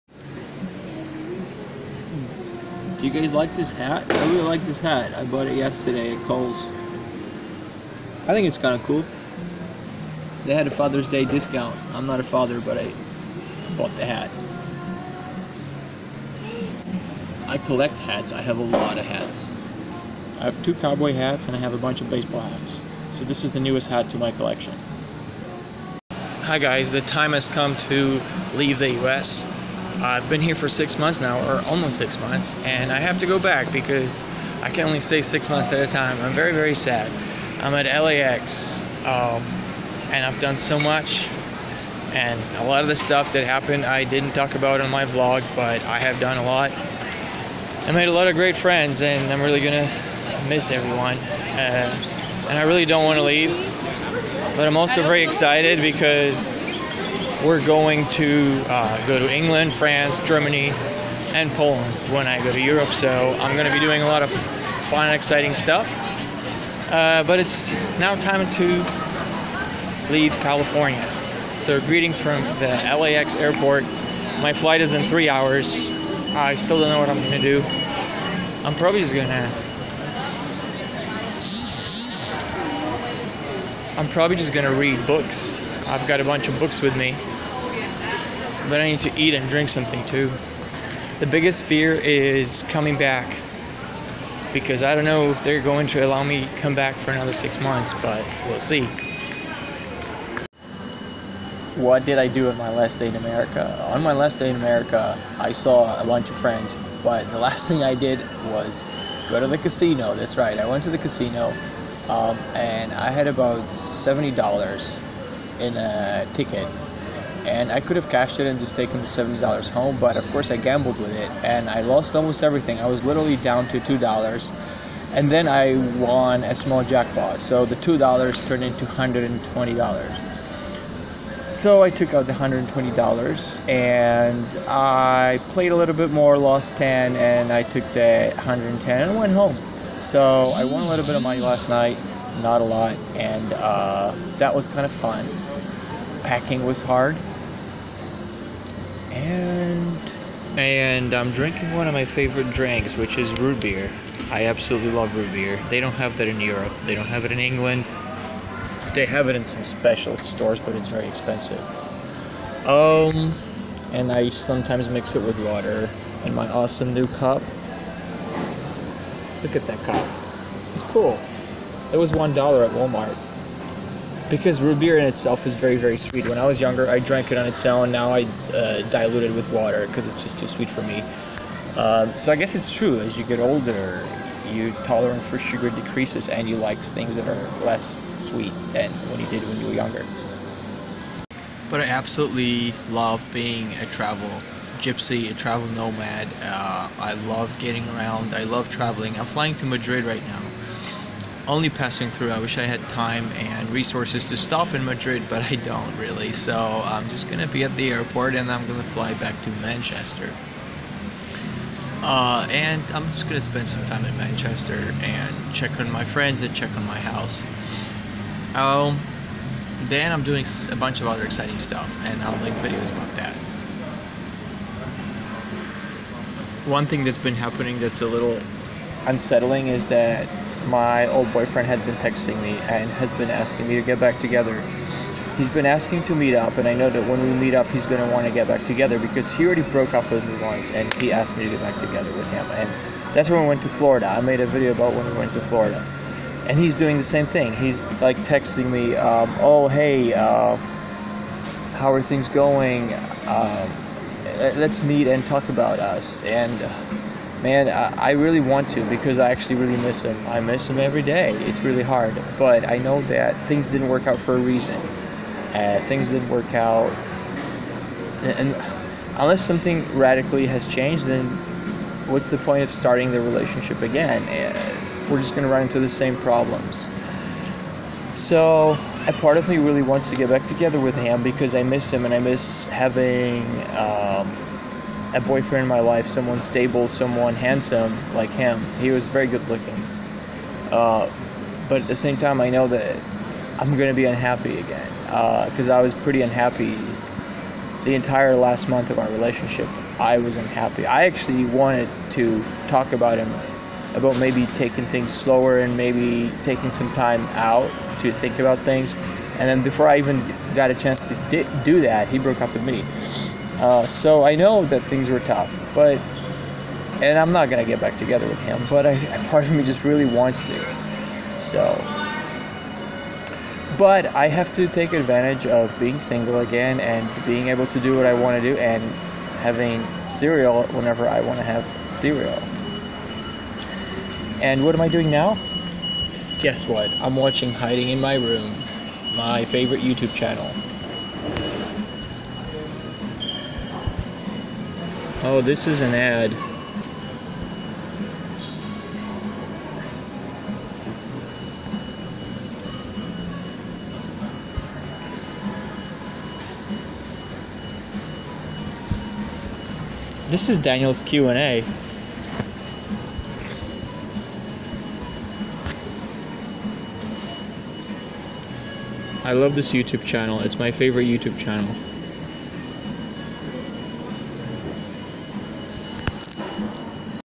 What did I do on my last evening in California, boyfriend wants to get back??
Update before I leave California for 4 weeks. I think my boyfriend wants to get back with me.